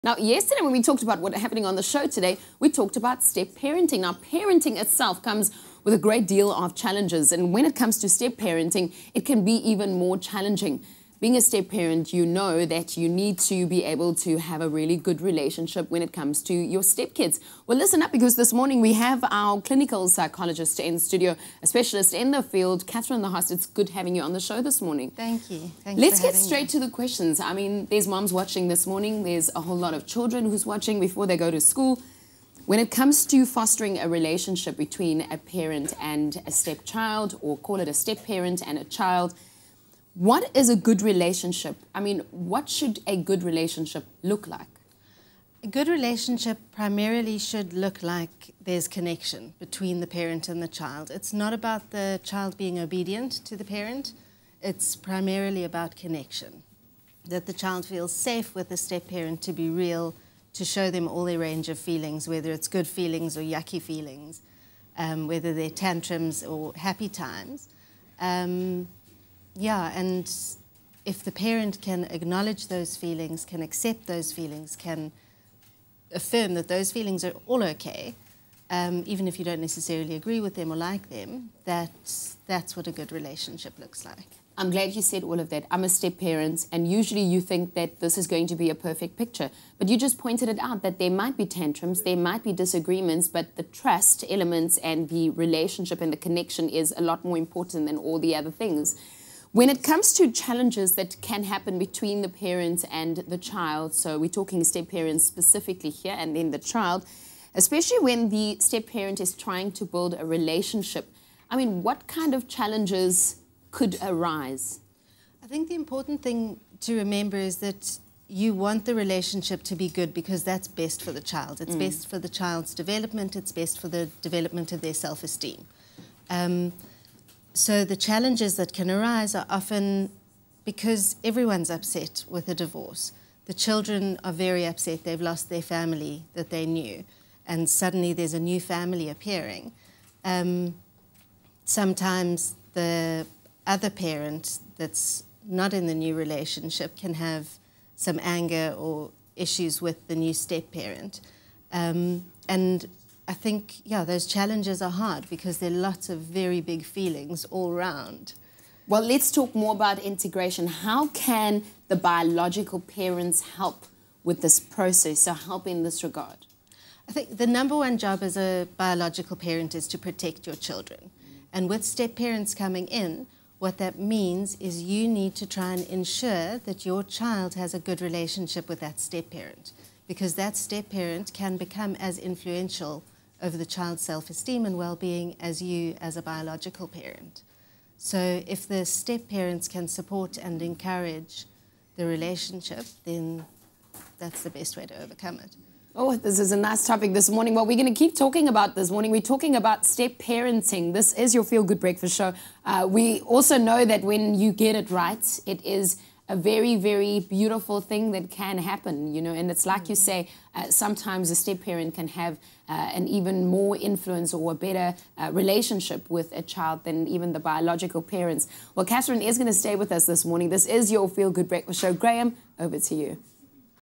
Fostering a Relationship Between Stepparent & Child 3/3
We chat to a clinical psychologist about fostering a good relationship between stepparent & child